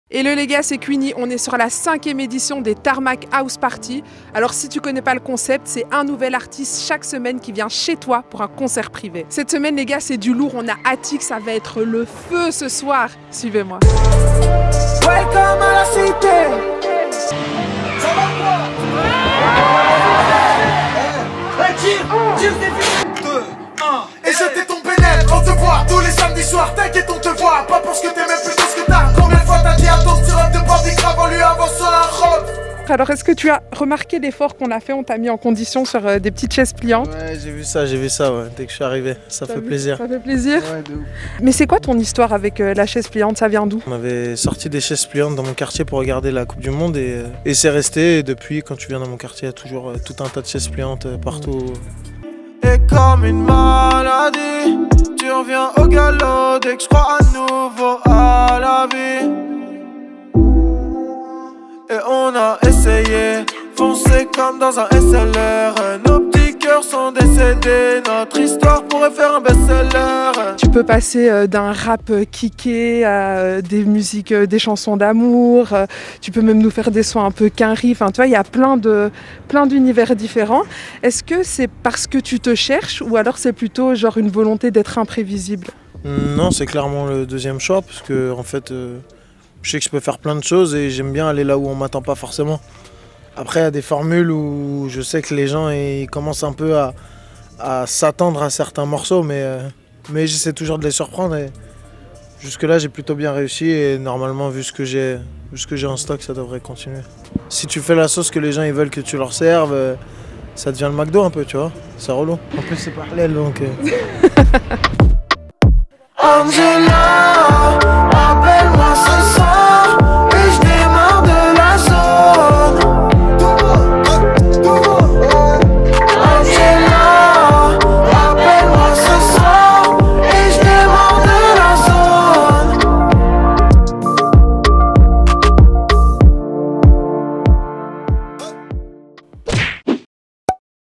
HATIK • TARMAC HOUSE PARTY
Queeny et la team sont partis chez Arno pour un mini-showcase d' Hatik pour lui et ses amis!
Et ça donne ça!

* Images tournées avant le Conseil National de Sécurité de ce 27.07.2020

Cette année, pas de festivals ...
Tarmac s'invite chez toi tous les week-ends de l'été pour des showcases 100% privés et exclusifs : les TARMAC HOUSE PARTY, le tout diffusé en live sur les réseaux de Tarmac 🔥